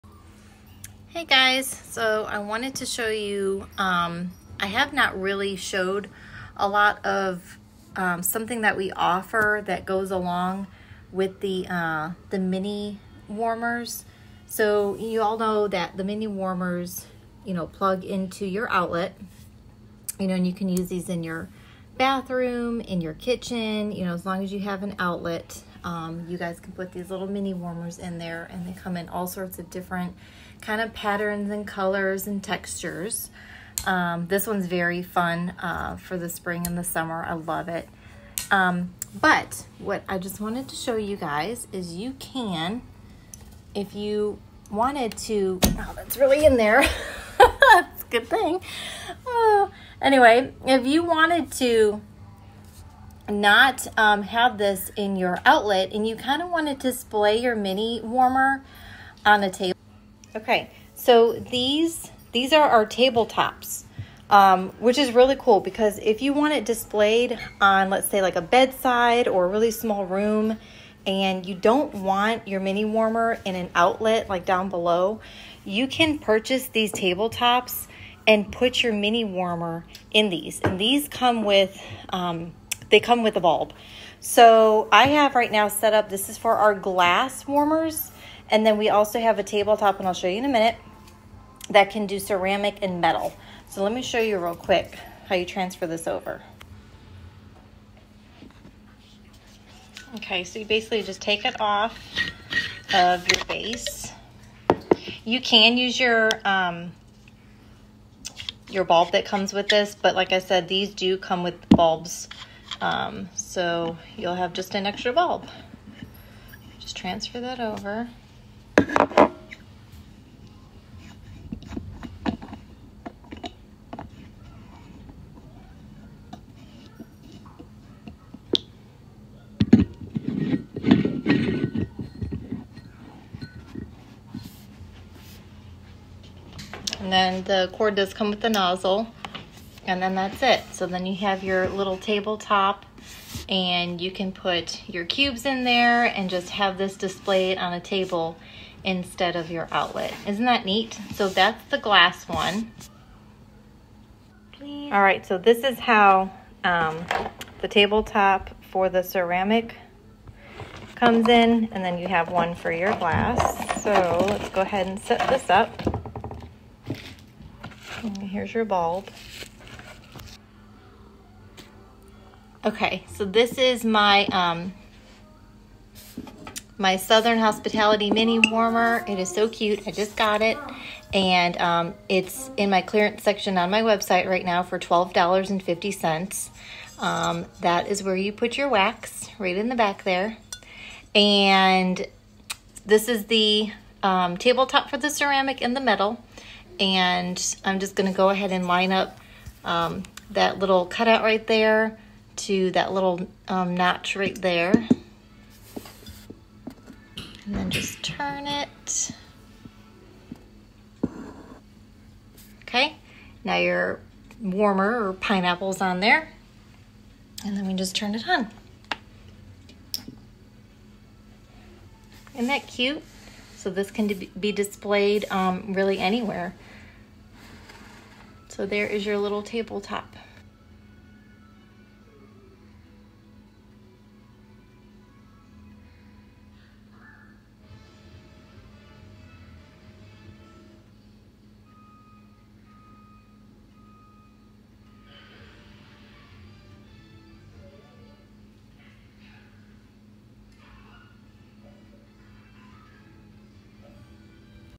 Scentsy | Tabletop base
Turn your mini warmer into a tabletop warmer! Love the versatility!

Join my VIP FB: making scents by Mia

Get Happy Mail!